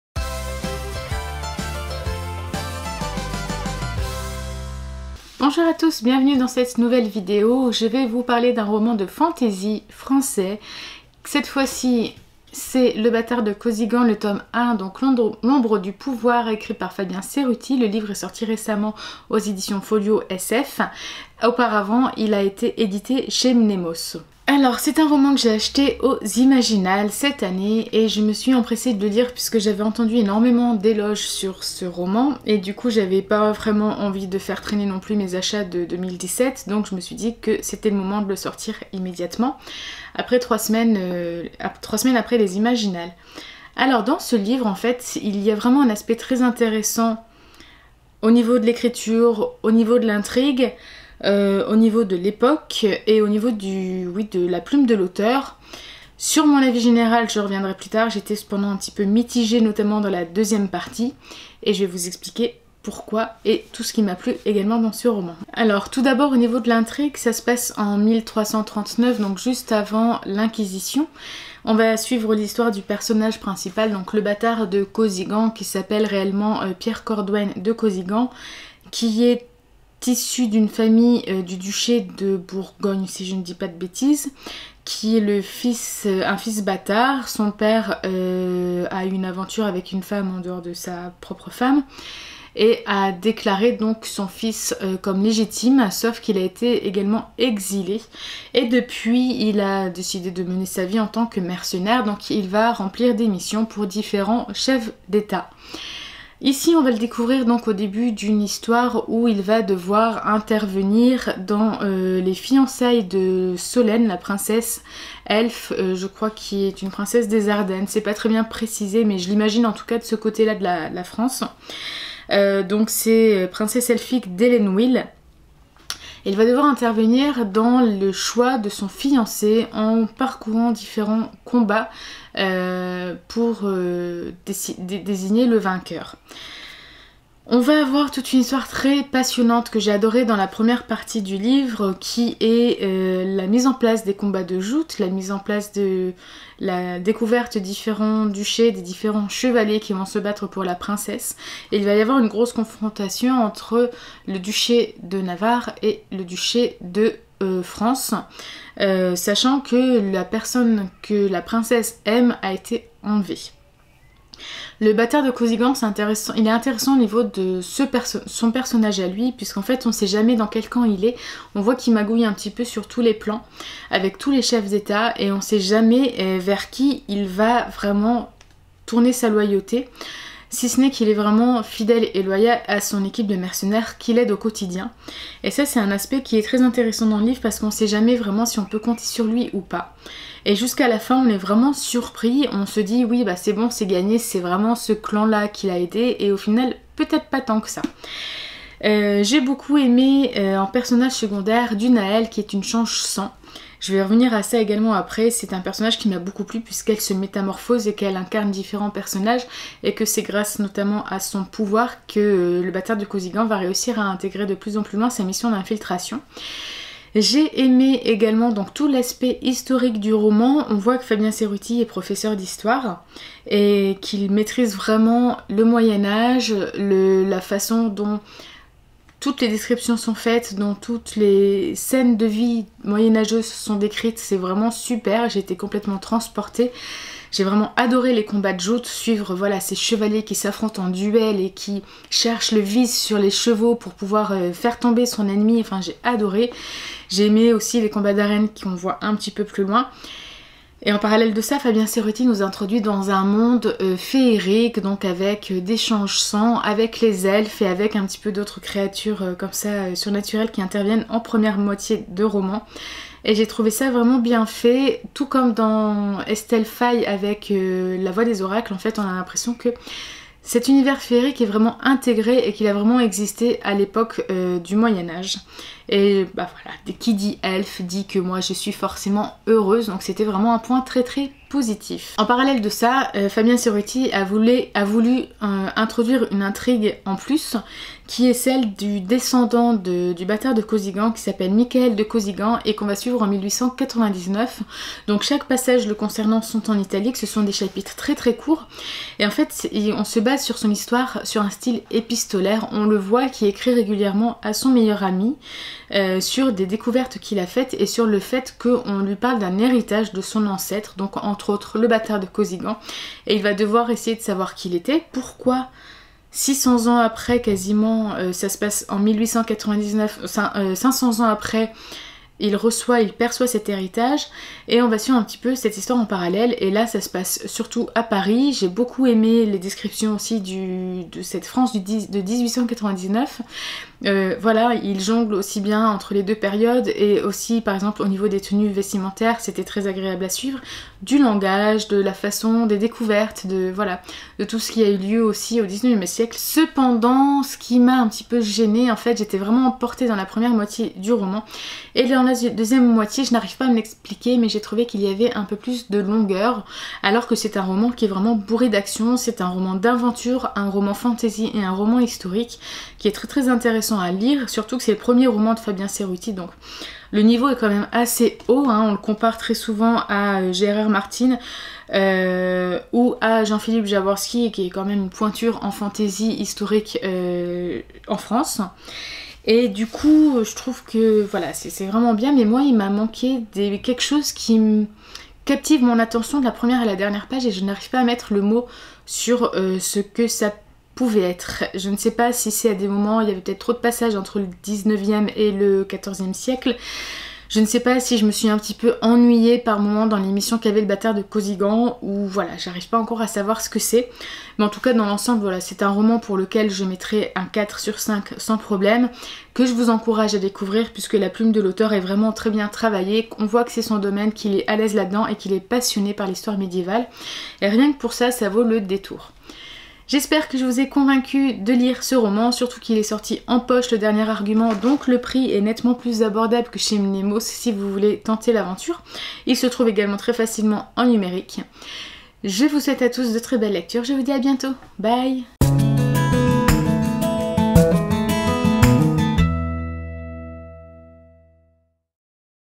FANTASY : LE BATARD DE KOSIGAN T1 : L'OMBRE DU POUVOIR DE FABIEN CERUTTI
Je vous retrouve pour mon avis sur ce superbe roman de fantasy : Le batard de Kosigan, t1 : l'ombre du pouvoir, de Fabien Cerutti

★ Me contacter

A bientôt!
Hélène